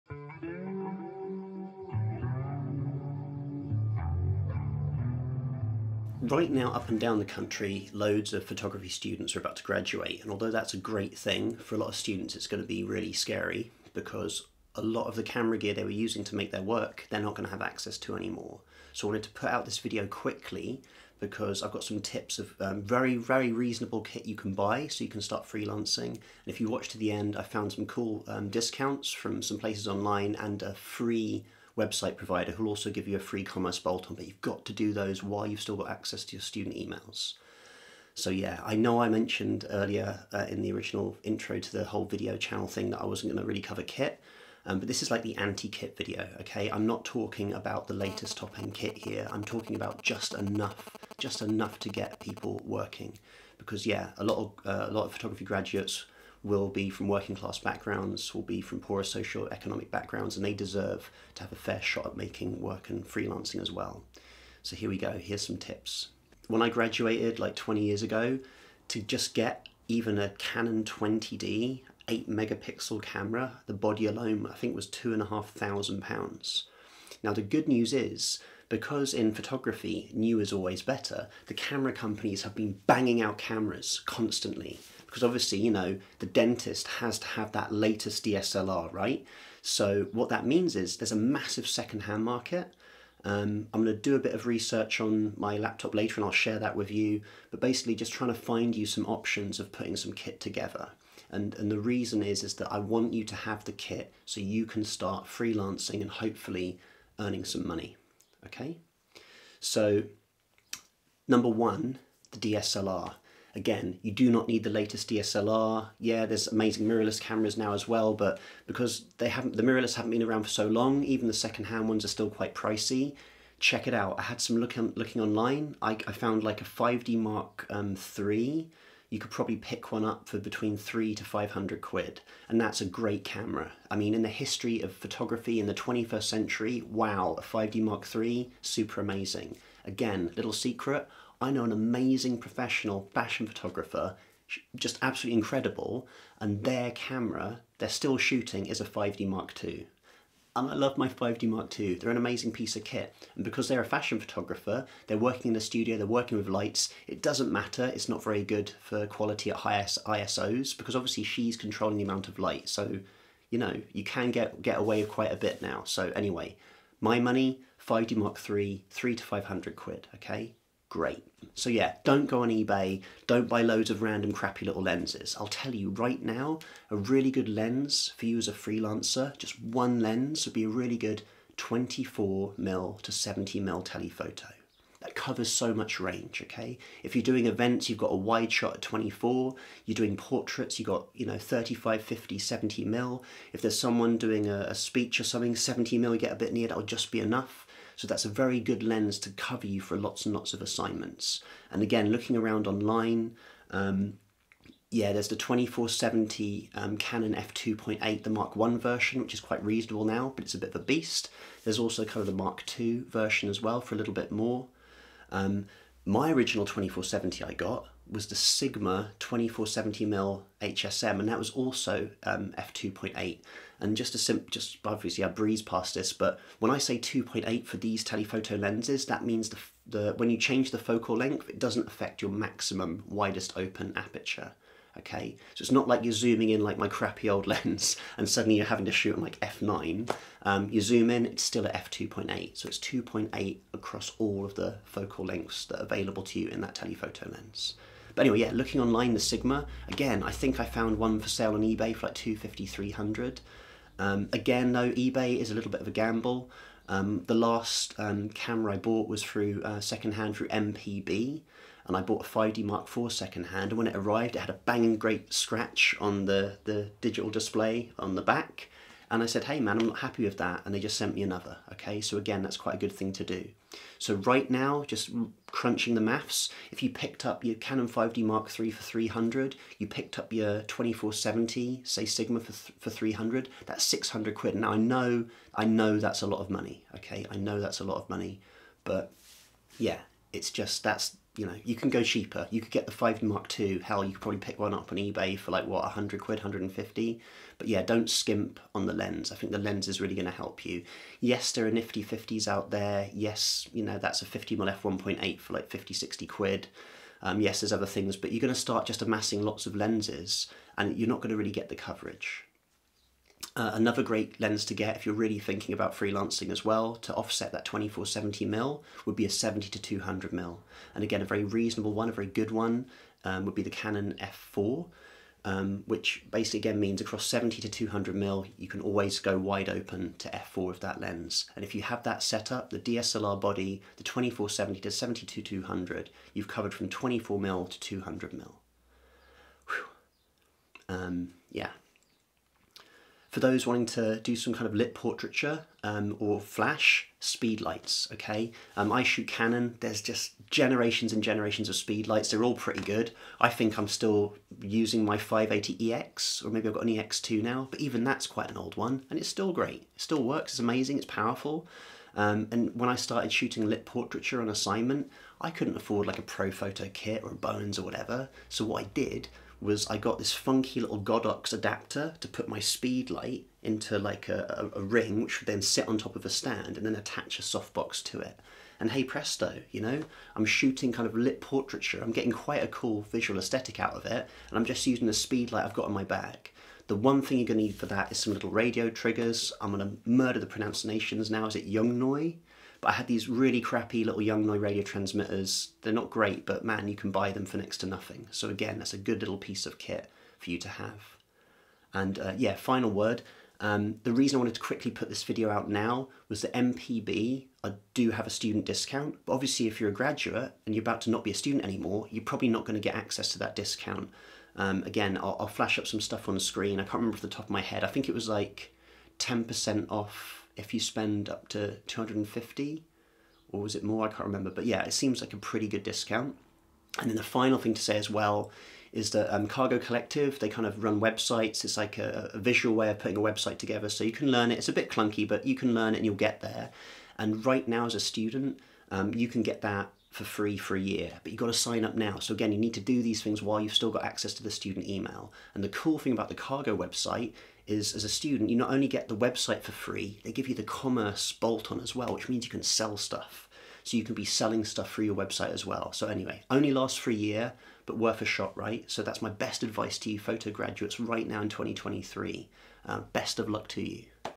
CHEAP camera gear for photo graduates! DSLR Body+Lens under £300. Budget kit options.
To all the 2023 graduates who are terrified about losing access to camera gear at university. don't worry. You can get some great kit for very little money.

In this video I cover some incredible budget kit options so you can get into freelancing as a photographer. I do focus mostly on Canon, as that's the kit I know and could show you myself. Obviously there are other camera brands out there and equivalent lenses for those cameras.

If you have any ideas for future shows please put suggestions in the COMMENTS below.